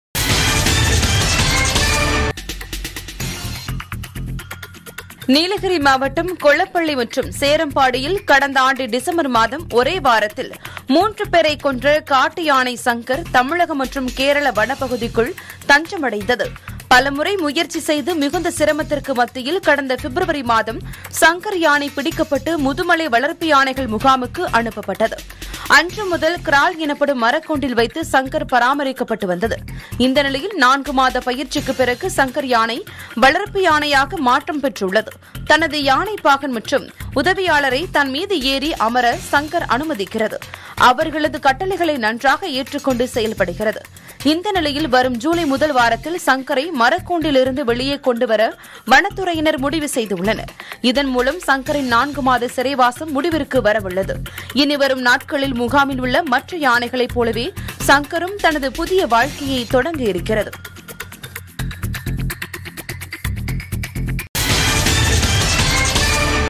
4 மாதத்திற்கு பிறகு சிறையை விட்டு வெளியே வரவுள்ள சங்கர் யானை! | செய்தித் துளிகள் | PuthuyugamTV
4 மாதத்திற்கு பிறகு சிறையை விட்டு வெளியே வரவுள்ள சங்கர் யானை! | செய்தித் துளிகள் | PuthuyugamTV